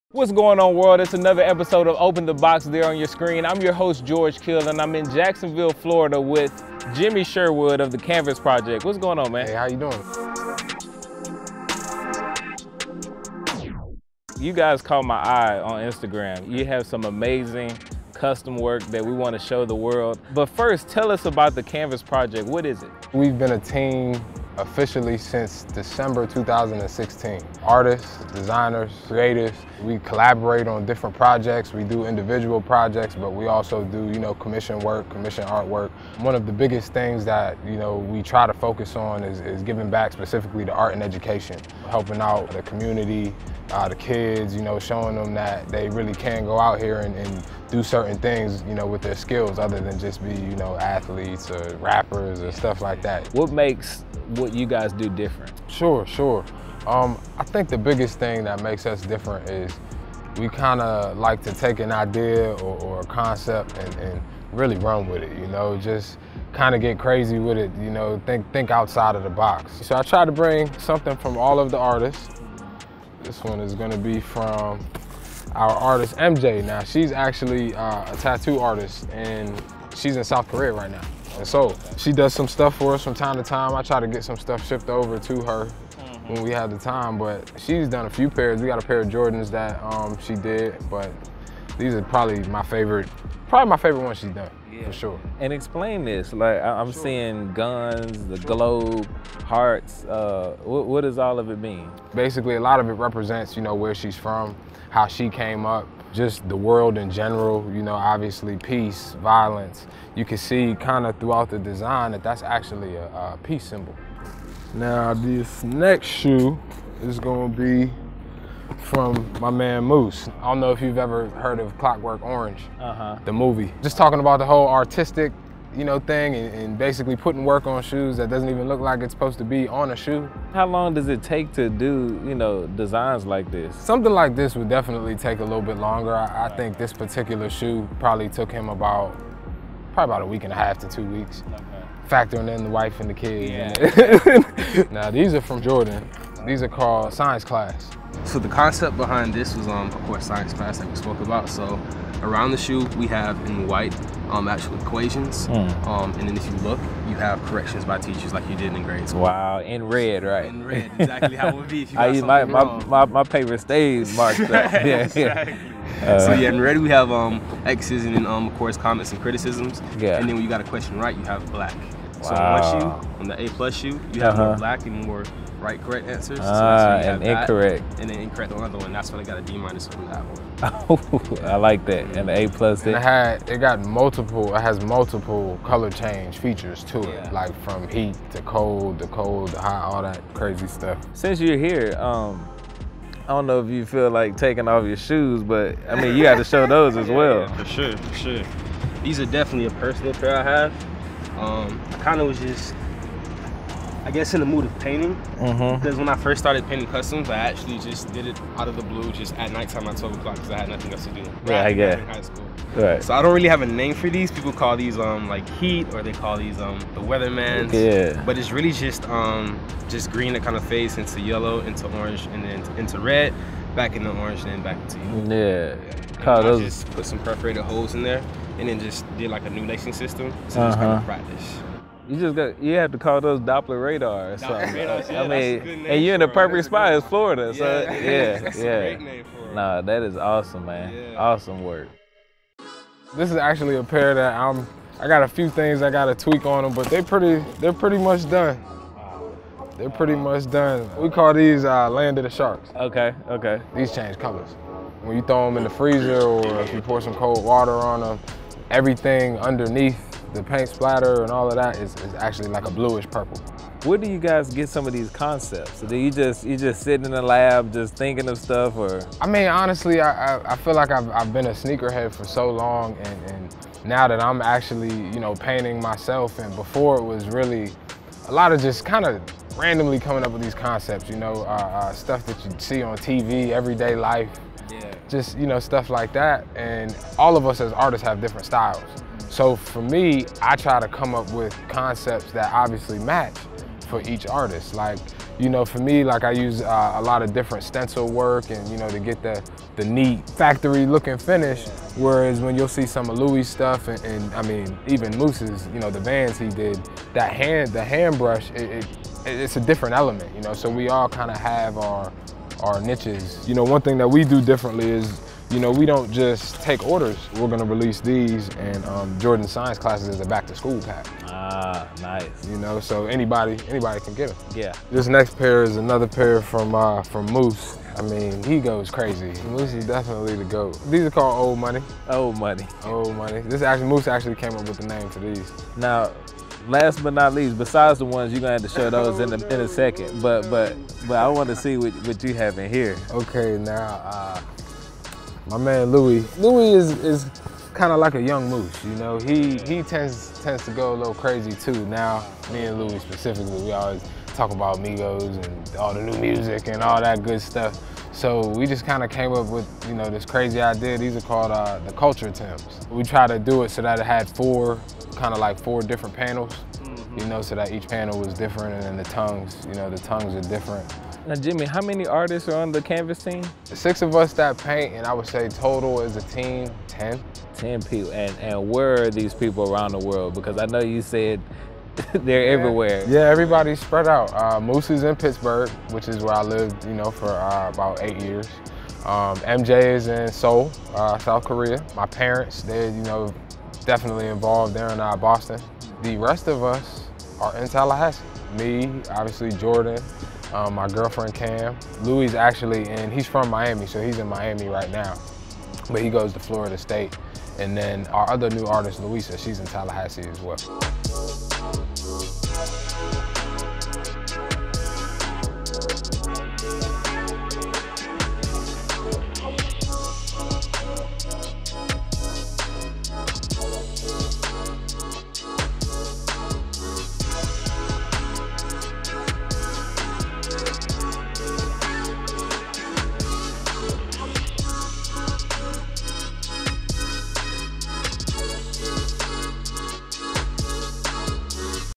the CANVAS Project Creates Custom Sneakers Unlike Any Other | Open the Box
Custom sneakers aren't new. With one Google search, you can find countless customizers showcasing their work on various shoe silhouettes; however, what the CANVAS Project is currently creating is truly unprecedented. Instead of fusing a ton of colors together, CANVAS is authentically fusing true art with customization, and their designs are brilliant. In our latest episode of Open the Box, we trek to Florida to understand what the CANVAS project is all about.